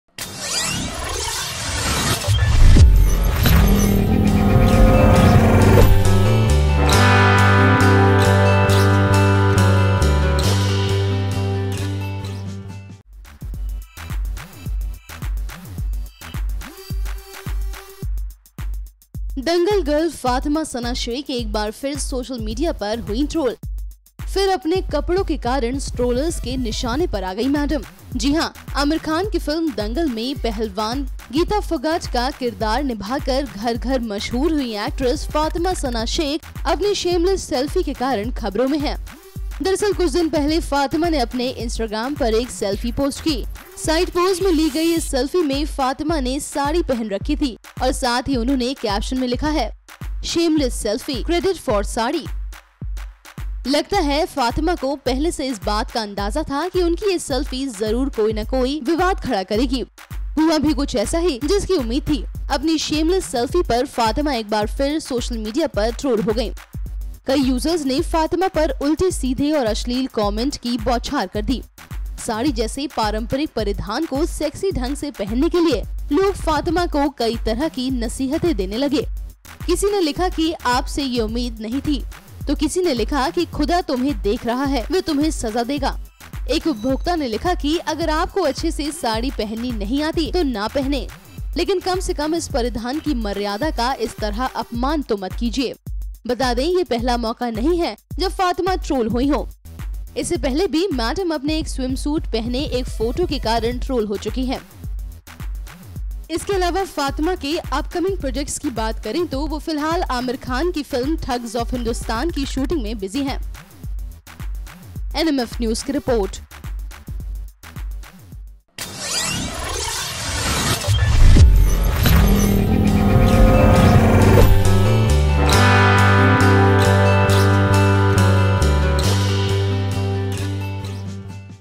Fatima Sheikh की Shameless selfie पड़ी उनपर भारी, Users ने किया TROLL
Actress Fatima Sana Sheikh has become famous for playing the role of Wrestler Geeta Fogat in the film ‘Dangal’ once again on the target of the trollers in the social media. In fact, Fatima had posted a selfie on Instagram three days ago.In this selfie taken at the side pose, she has worn some sari in this way, in which a portion of the neck towards the waist and blouse is visible.